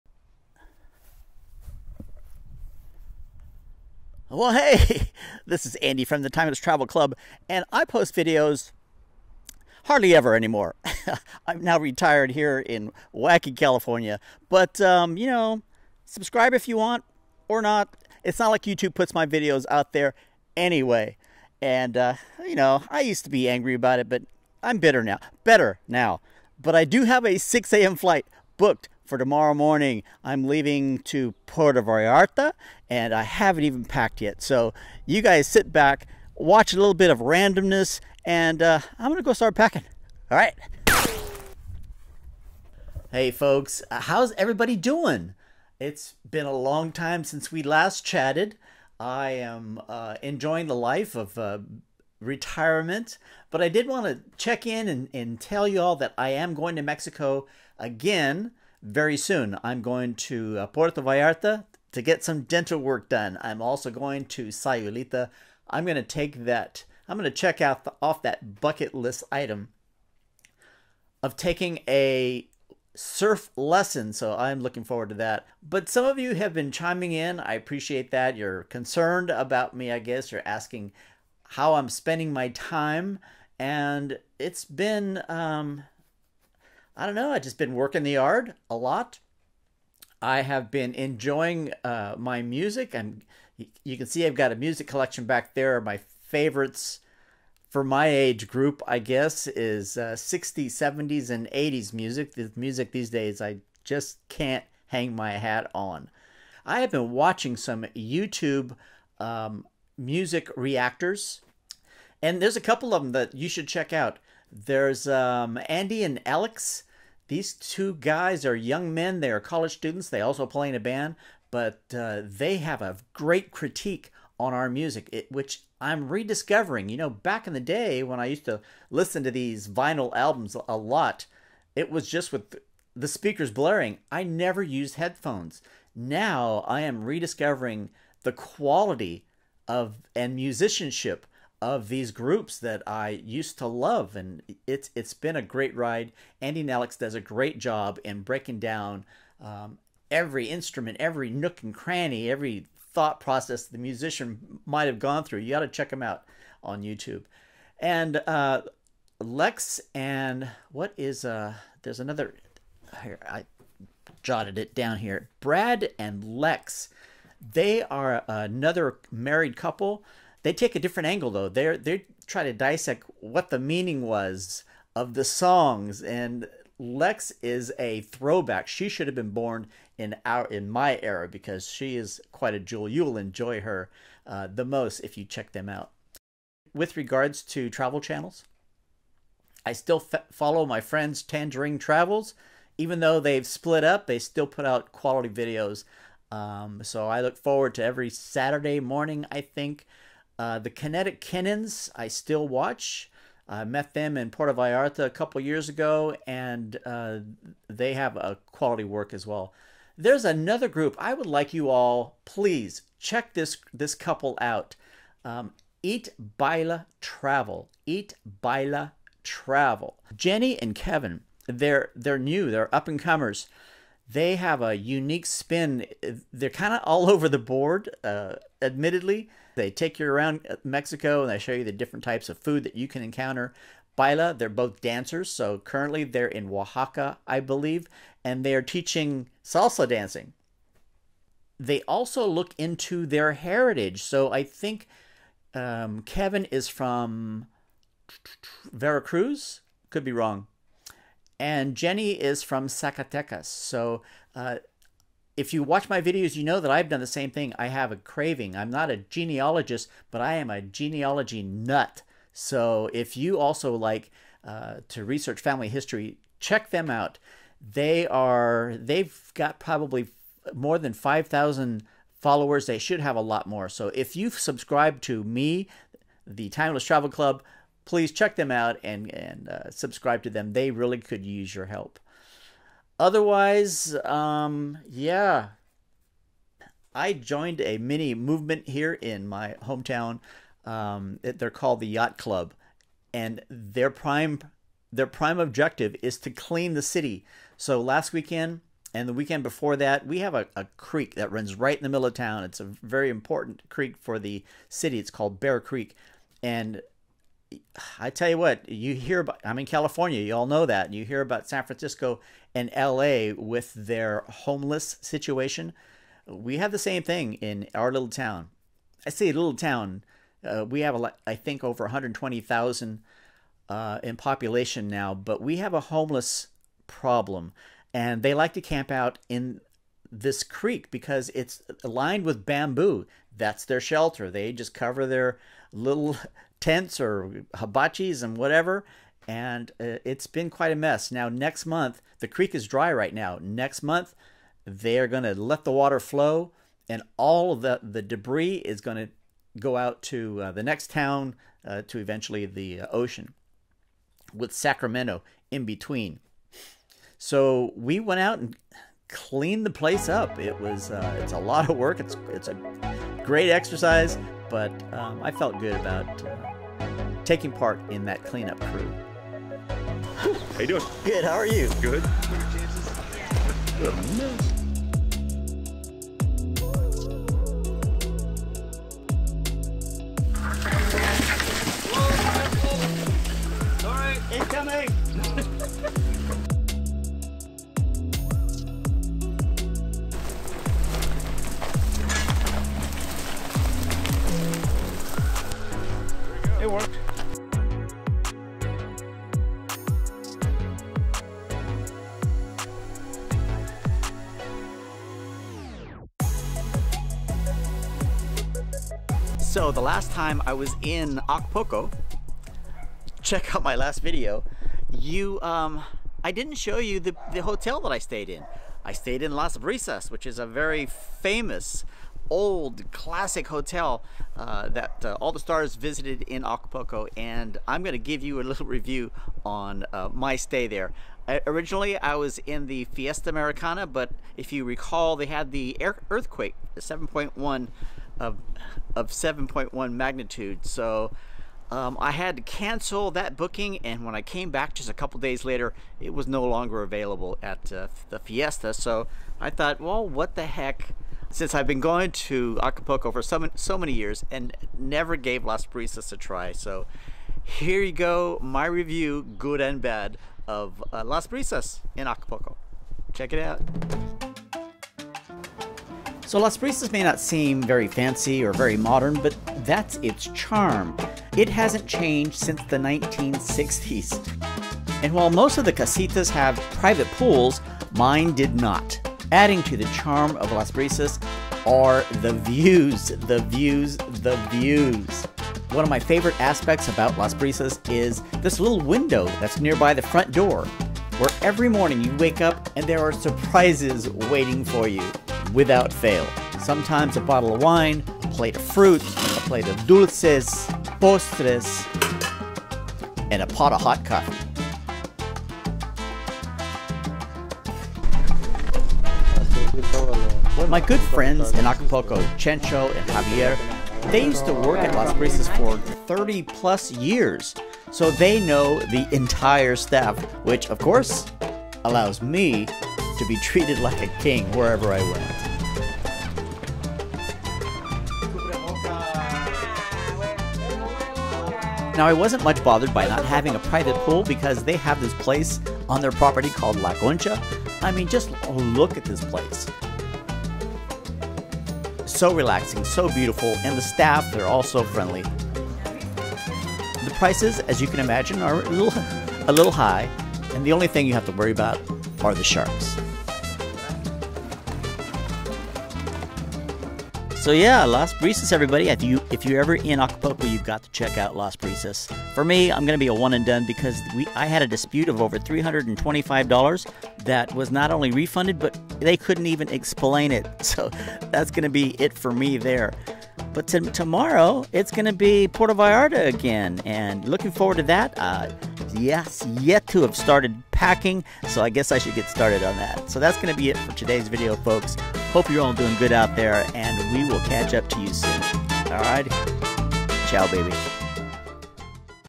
Just Checking In - How this ex-YouTuber is enjoying retirement.
By special request. Since traveling full time is now less possible, I am just checking in to share what I have been up to as well as where I am going.